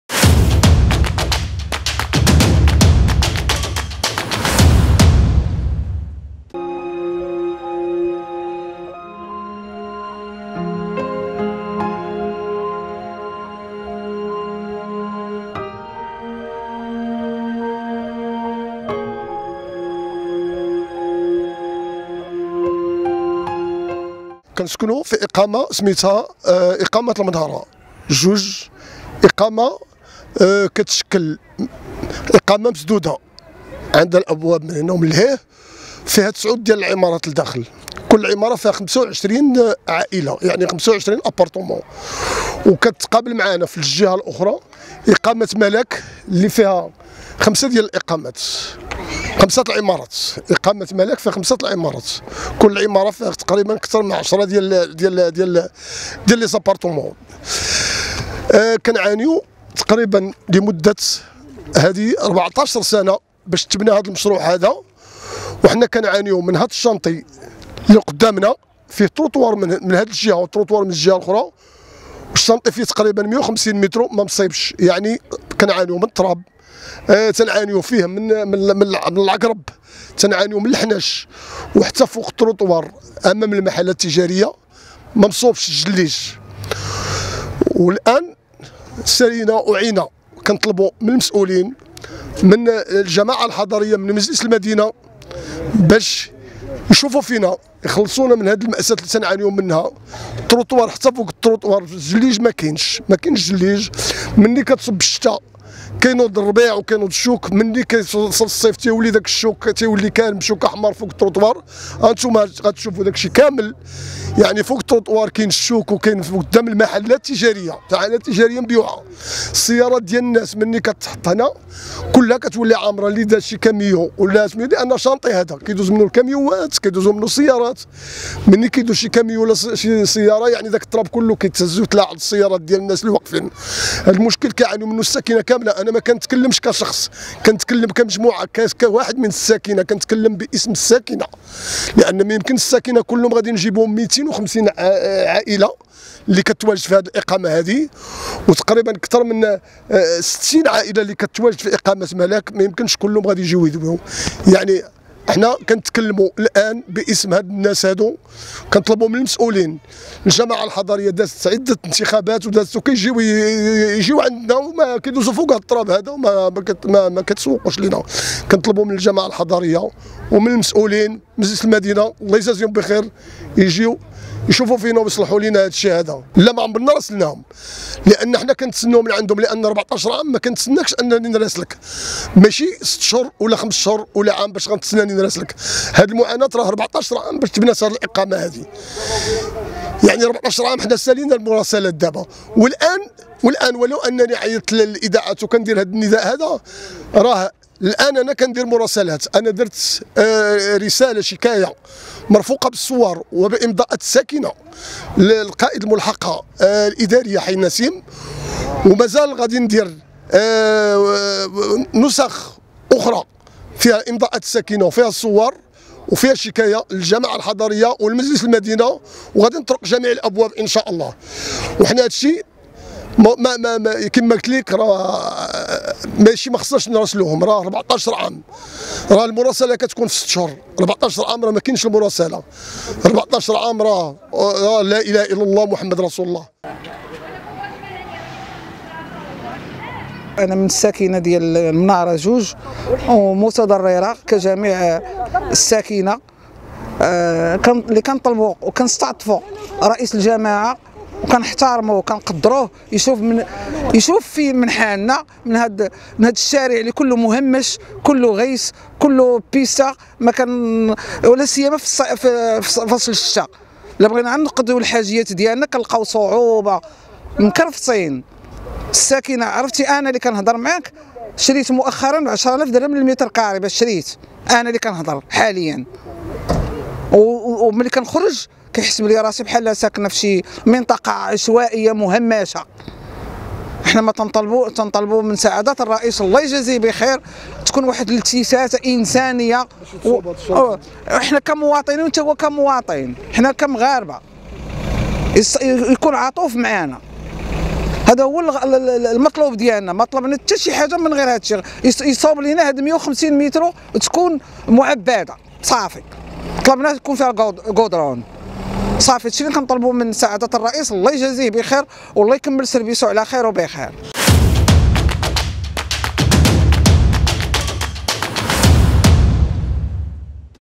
البيضاء: أنقذوا 300 أسرة من الغيس والغبار والعقارب بحي النسيم !
سكان حي النسيم يناشدون مجلس المدينة لإتمام أشغال شوارع اقامة المنارة 2 حي النسيم بالبيضاء

أخبار المغرب
# أخبار عاجلة